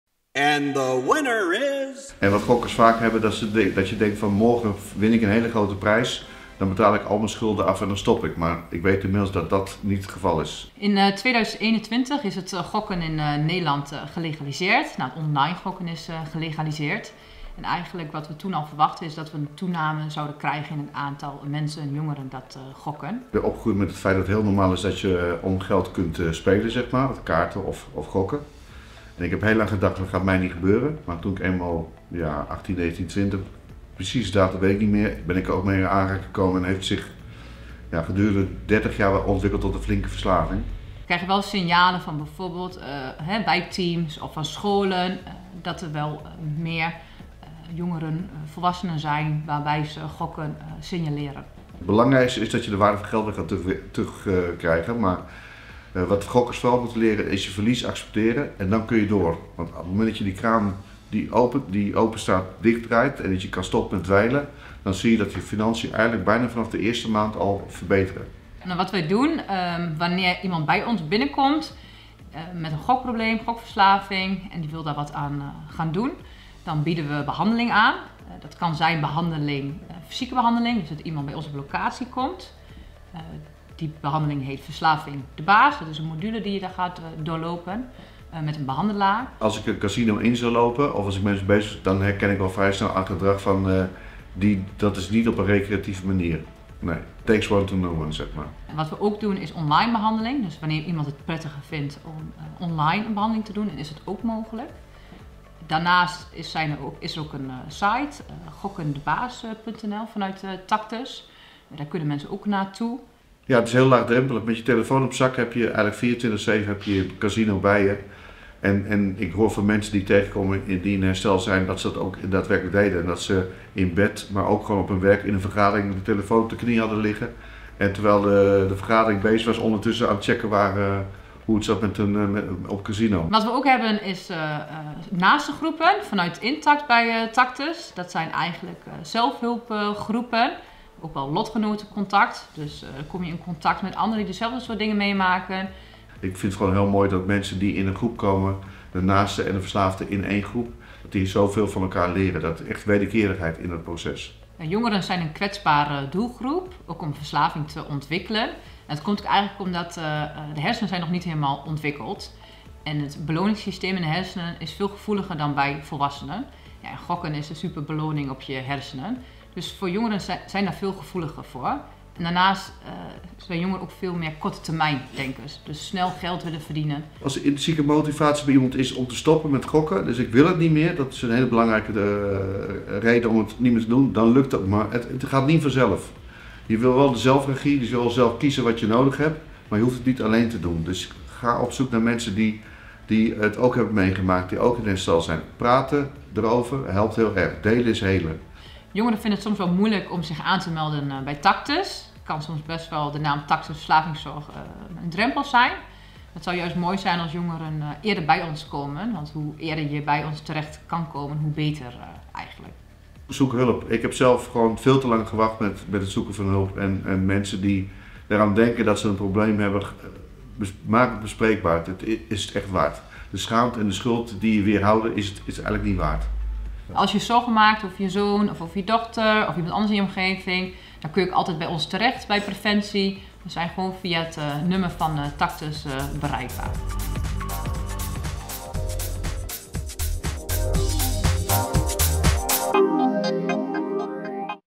Tactus ziet toename in gokkers
Nadat online gokken legaal werd in Nederland nam het aantal gokverslaafden ook flink toe.